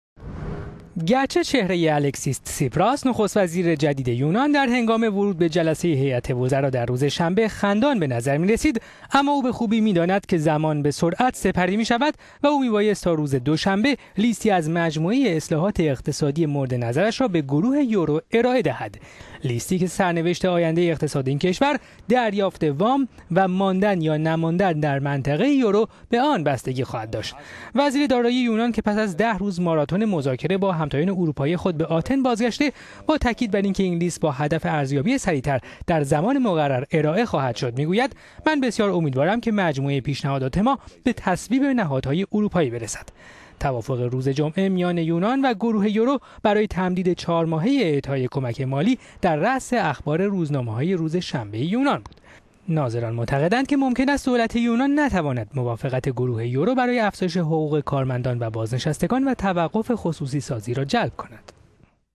ابراز امیدواری وزیر دارایی یونان به تصویب اصلاحات پیشنهادی توسط نهادهای اروپا
گرچه چهره الکسیس تسیپراس، نخست وزیر  جدید یونان، در هنگام ورود به جلسه هیئت وزرا در روز شنبه، خندان به نظر می رسید، اما او به خوبی می داند که زمان  به سرعت  سپری می شود و او می بایست تا  روز دوشنبه، لیستی از مجموعه اصلاحات اقتصادی مورد نظرش را به گروه یورو ارائه دهد.

لیستی که سرنوشت آینده اقتصاد این کشور، دریافت وام و ماندن یا نماندن  در منطقه یورو، به آن بستگی خواهد داشت.

یانیس واروفاکیس، وزیر دارایی یونان که پس از ده روز ماراتن مذاکره با همتایان اروپایی خود به آتن بازگشته، با تاکید بر این که…